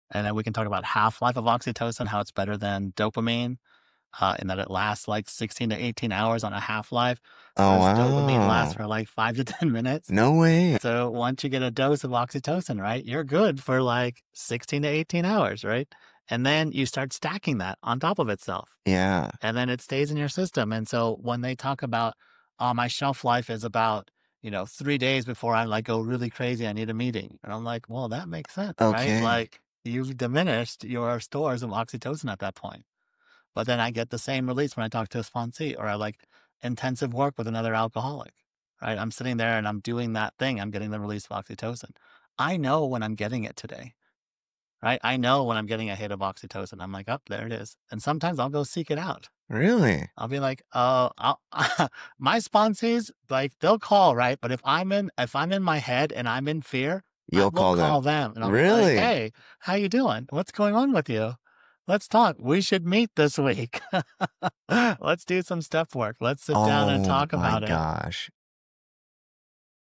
Why oxytocin beats dopamine every time
Dopamine fades in minutes… but oxytocin lasts up to 18 hours. Dr. Shah explains why this “love hormone” packs a bigger punch.